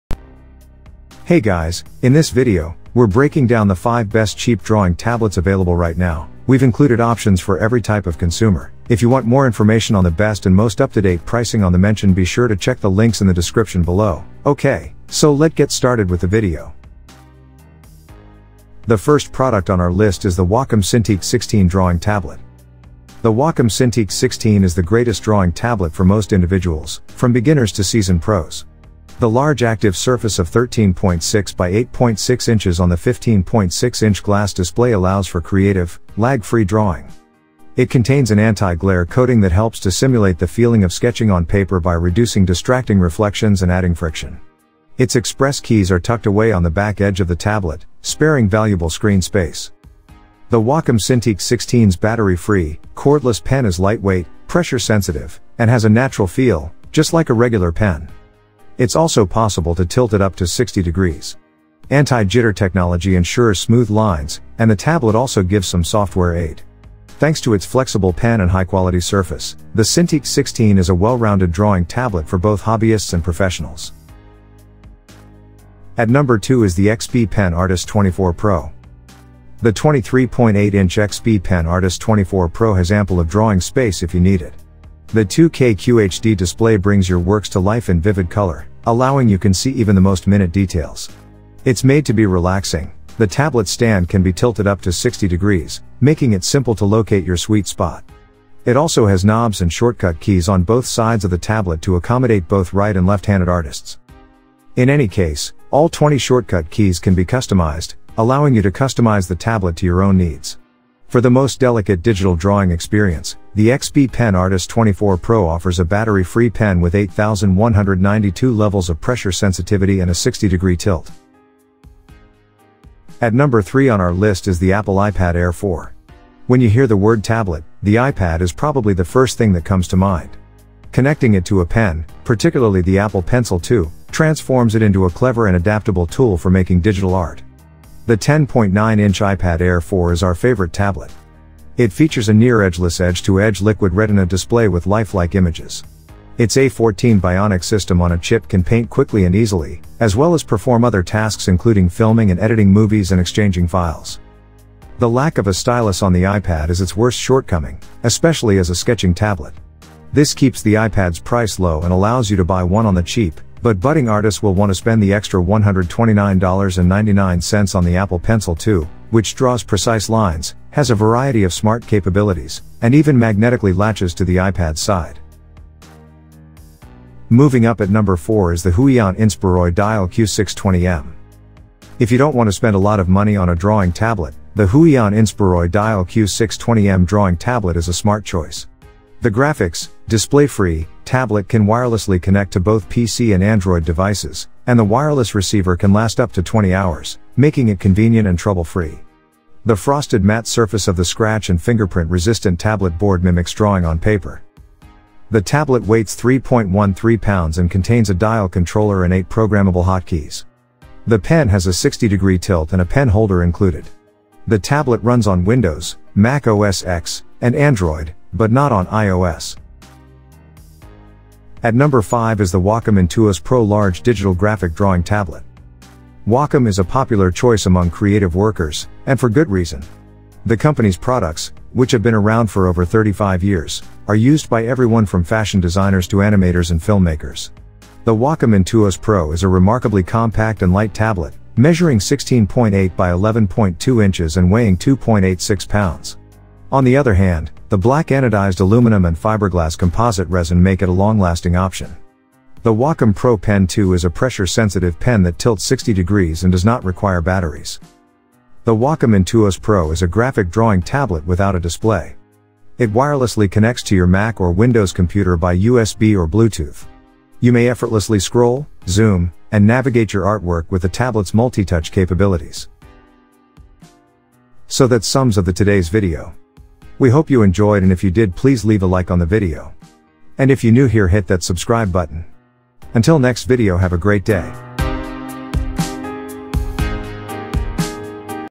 Quality and Savings: The Top Cheap Drawing Tablets of the Year 2023
► Links & Timestamps:

00:00​ Intro
1️⃣
00:22 XPPen Artist 12 Graphic Tablet Drawing.

2️⃣
01:20 XPPen Artist Pro 16 Graphic Tablet Monitor.

3️⃣
02:06 Xppen Artist Pro 16 Grafische Tablet Monitor.

4️⃣
03:01 GAOMON PD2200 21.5 Inches 92%NTSC Graphic Drawing Tablet.

5️⃣
03:45 XPPen New Artist 12 Pen Graphic Tablet Monitor Drawing Tablet.